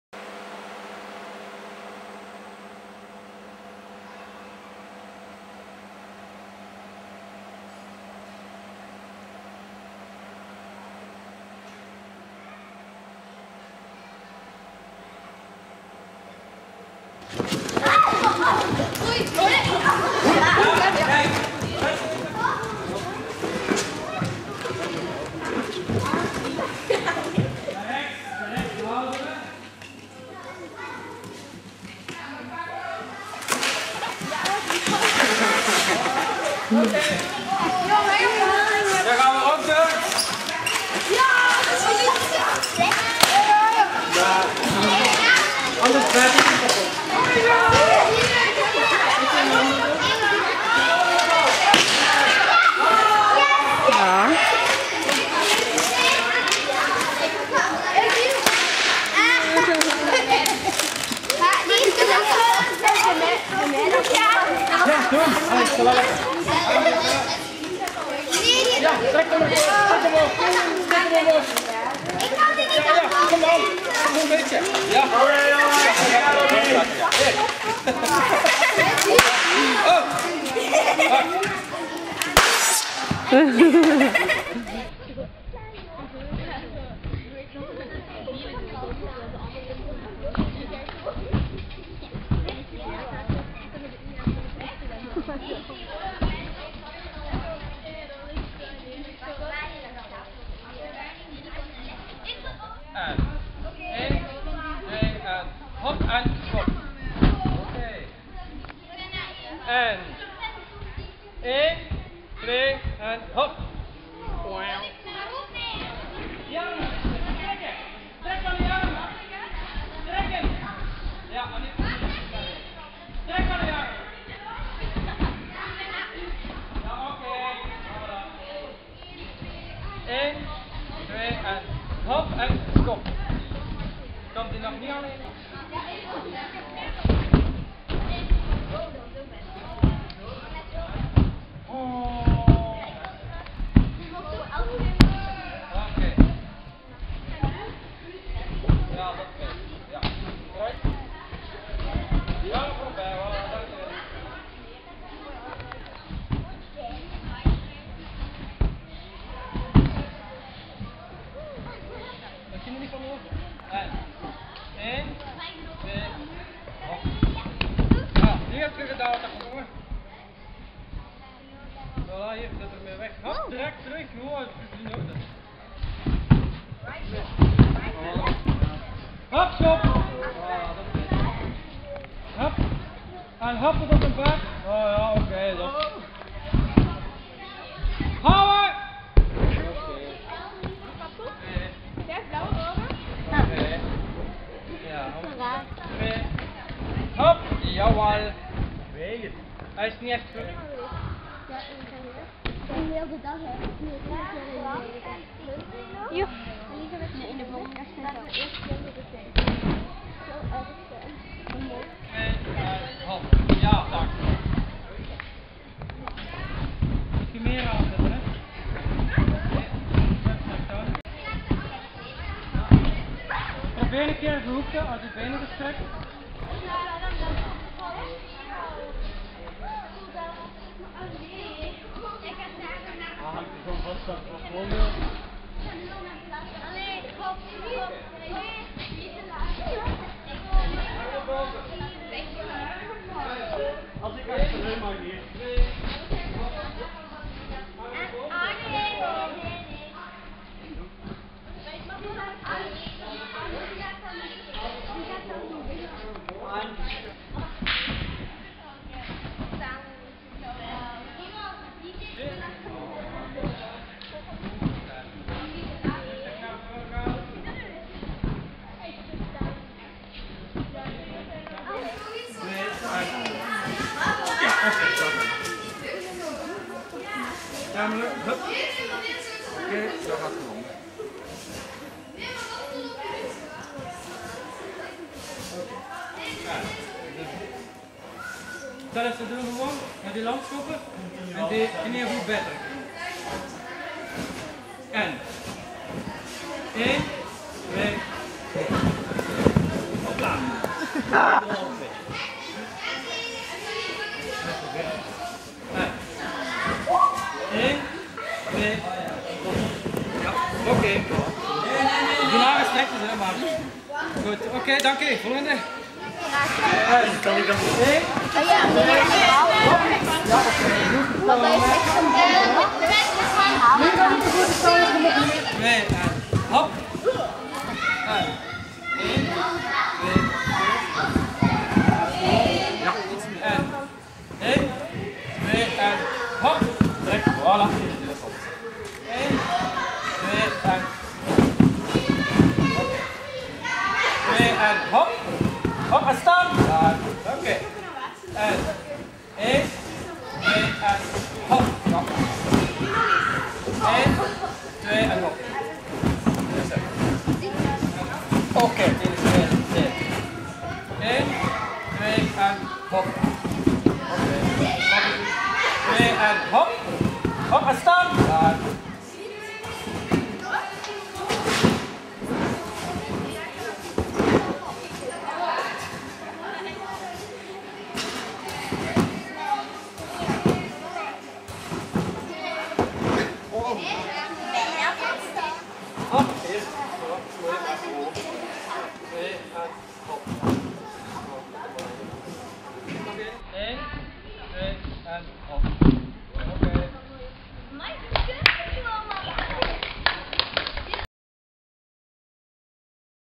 Trampoline kamp 2007
Leren springen op de trampoline bij Ragna en Petje.
In de turnclub.